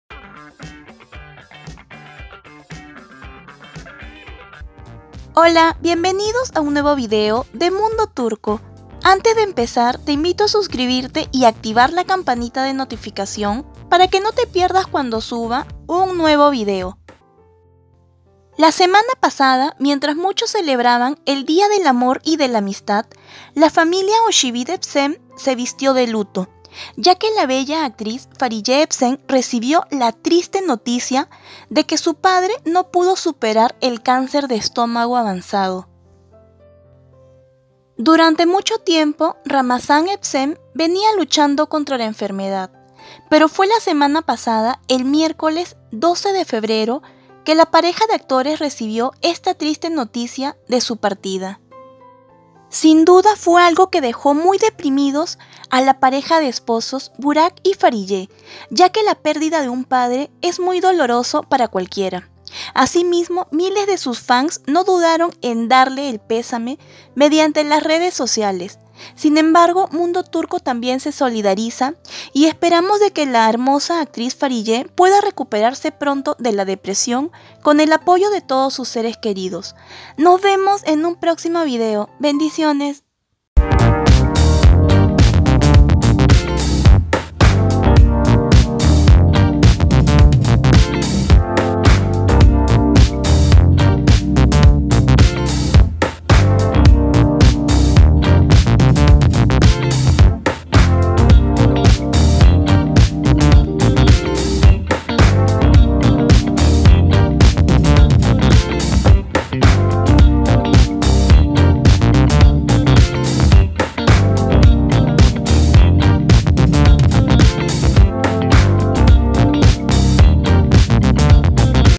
Fahriye Evcen Un Día Muy Doloroso
La Familia Ozcivit Evcen pasa por un mal momento